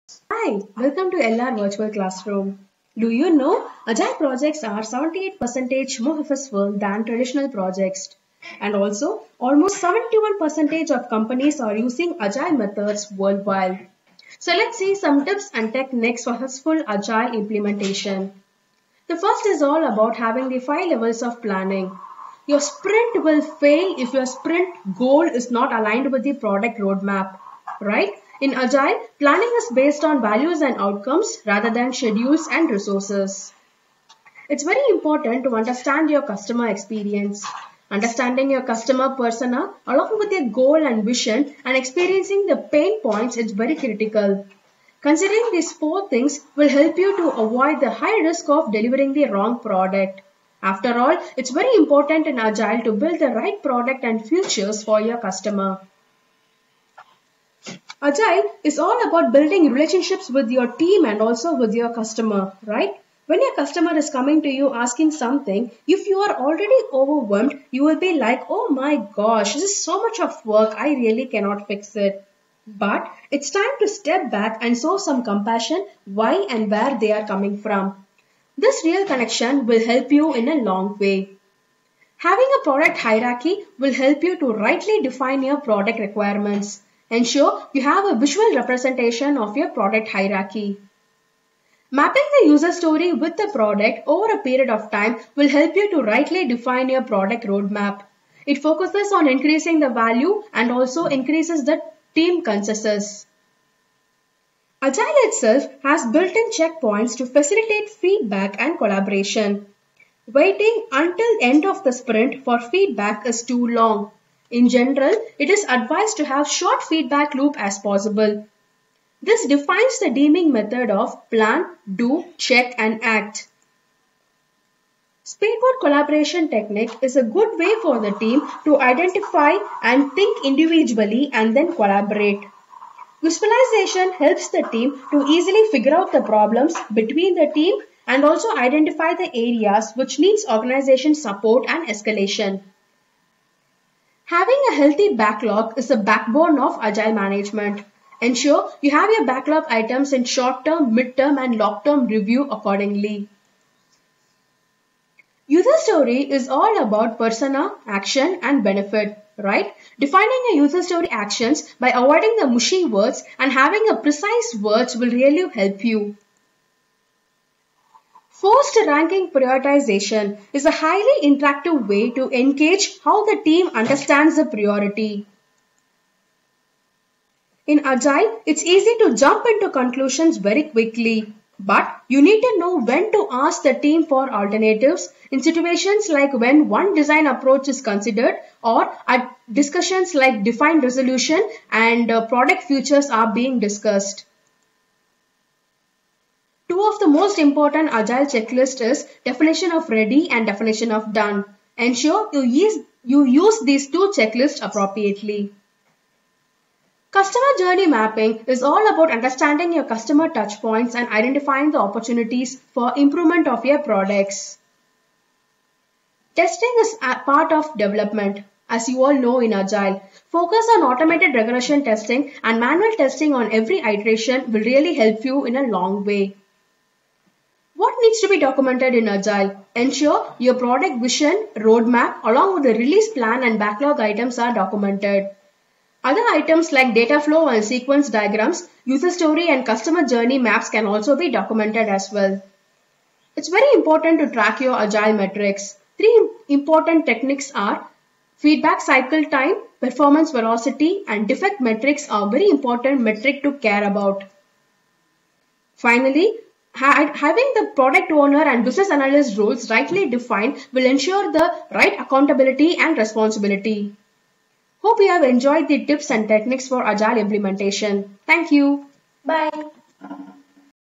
Introduction to Agile| Agile Best Practices| Agile Implemention Tips
In this video, we are going to review 15 various agile tips and agile techniques for successful implementation of Agile project methodology.

Techniques like speedboat collaboration, user story mapping, forced matrix prioritization are discussed in detail.

Office timeline - Create project timelines and roadmaps in 10 min